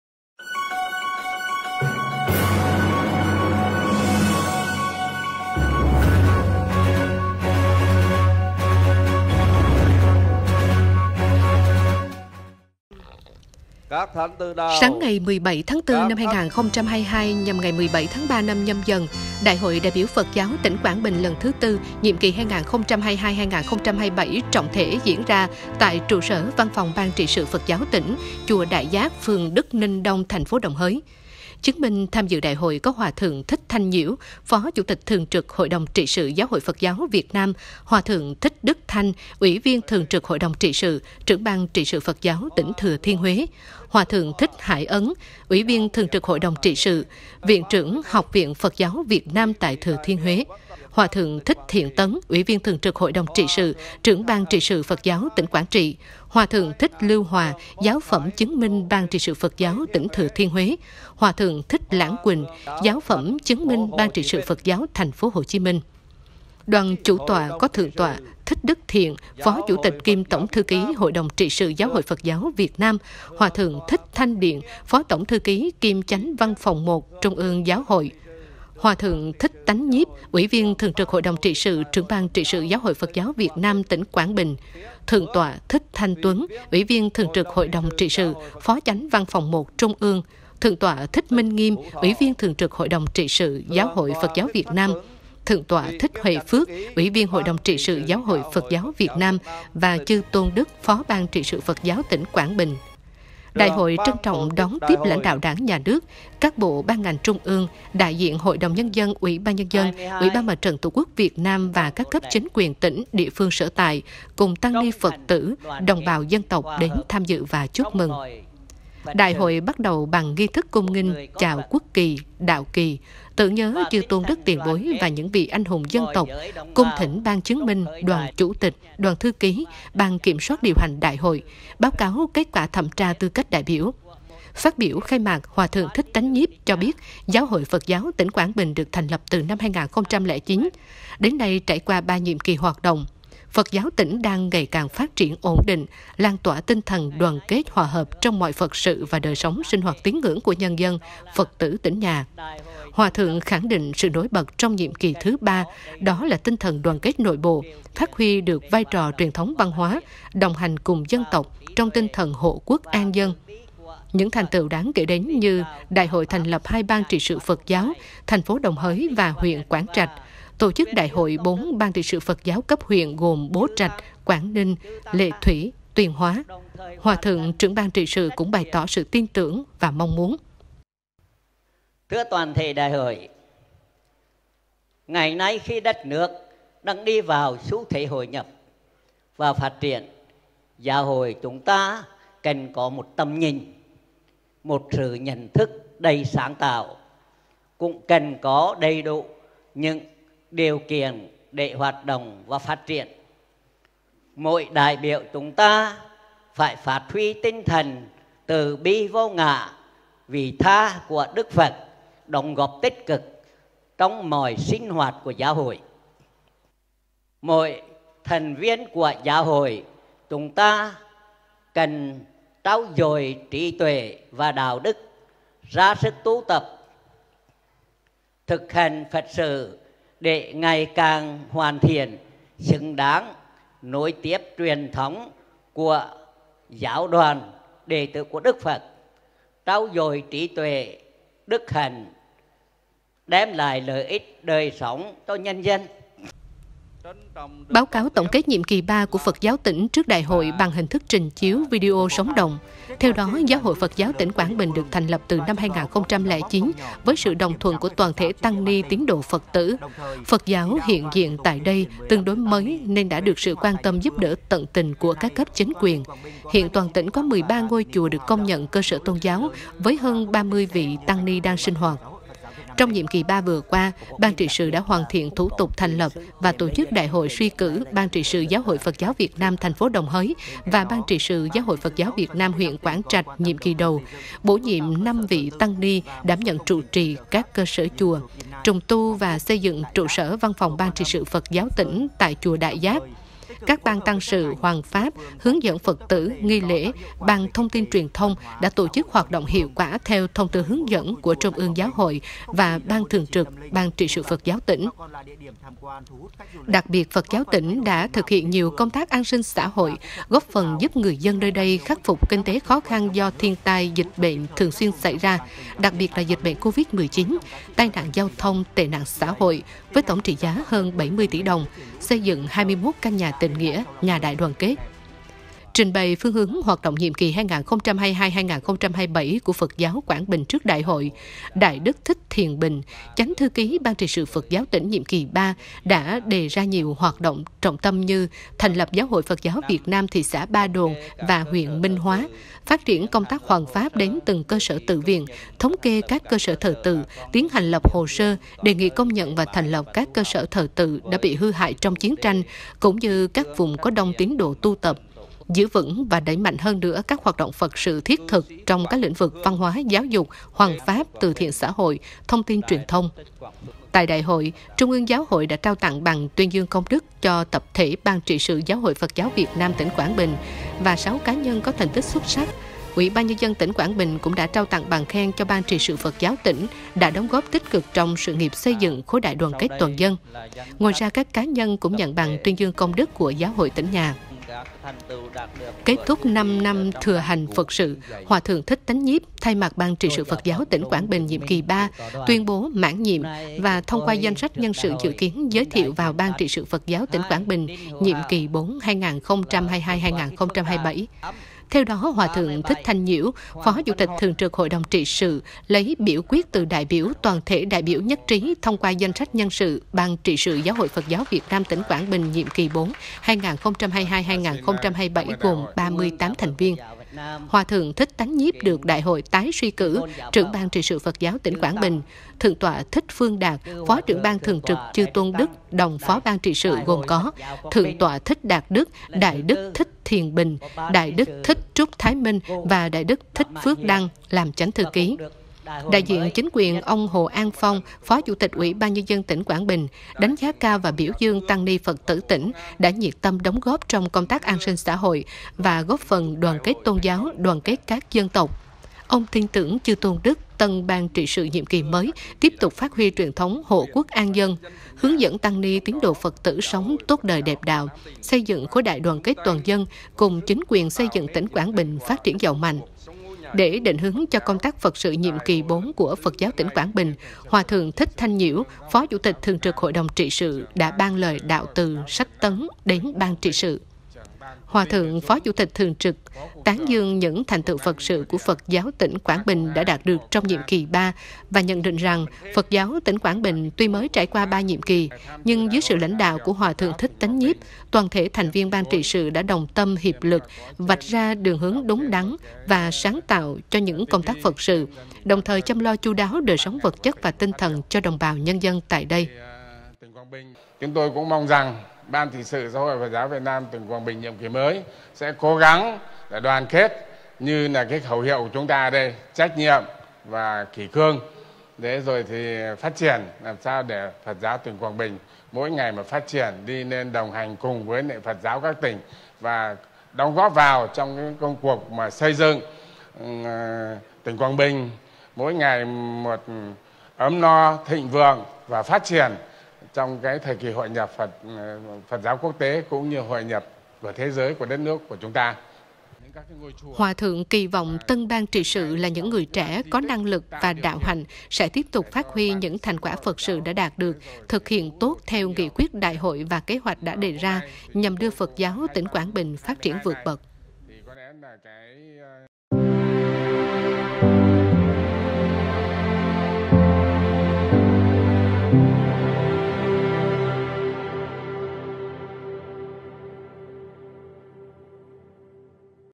Trọng thể Đại hội đại biểu Phật giáo tỉnh Quảng Bình lần thứ IV, nhiệm kỳ 2022 - 2027
Sáng nay ngày 17/4/2022 (nhằm 17/3 Nhâm Dần), Đại hội đại biểu Phật giáo tỉnh Quảng Bình lần thứ IV, nhiệm kỳ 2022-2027 trọng thể diễn ra tại tại Trụ sở Văn phòng BTS PG tỉnh (chùa Đại Giác, phường Đức Ninh Đông, TP. Đồng Hới).